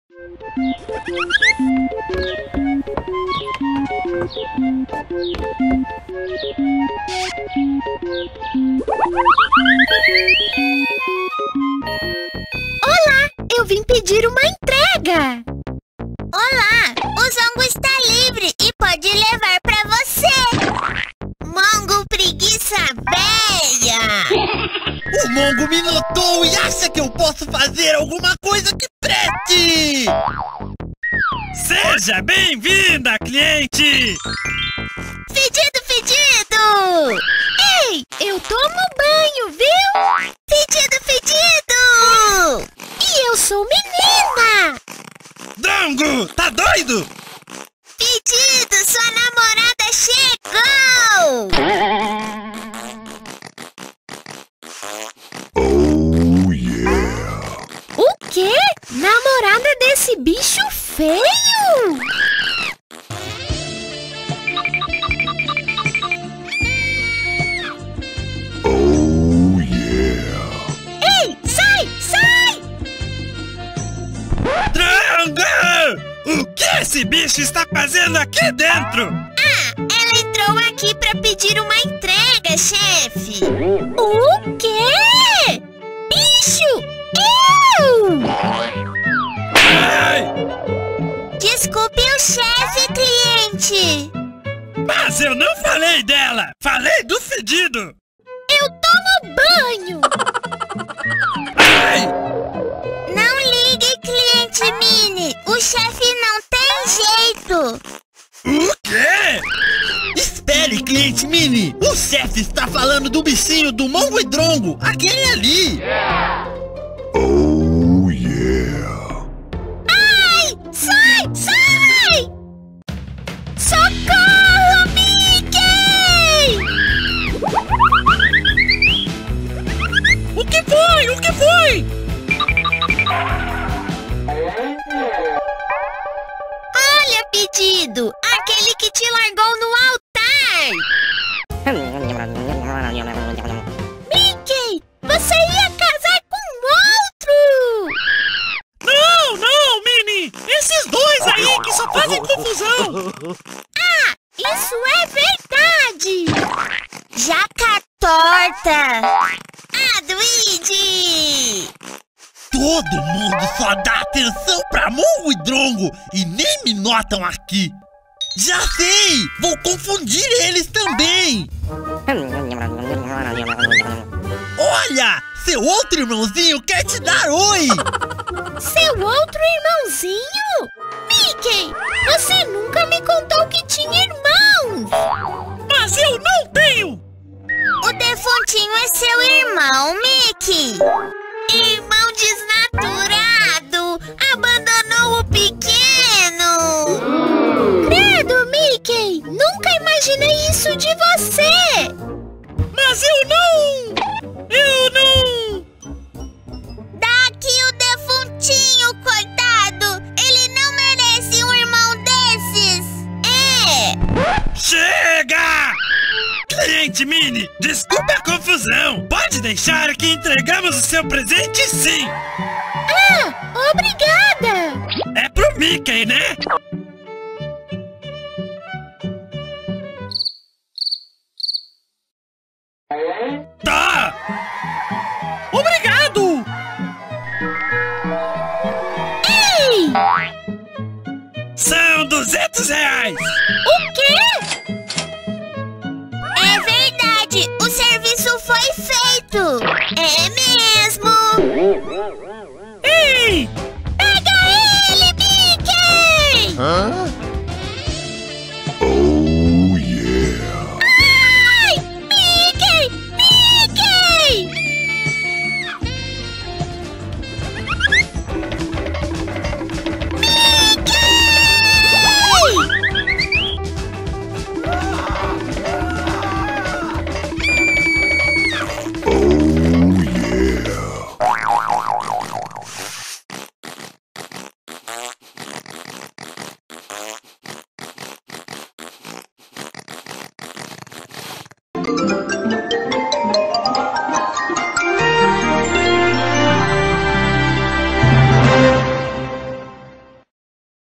Mongo e Drongo não mimam a Minnie - Desenho animado com Mongo e Drongo mendigos e Fedido paquerador
Mongo e Drongo mendigos recebem a visita de Minnie no trabalho deles. Ela quer enviar uma encomenda pra Mickey, mas o Fedido ao vê-la quer namorar com a coitada. Desenho animado com Minnie, Mongo e Drongo e sua turma: Fedido, Chefe e Zongo.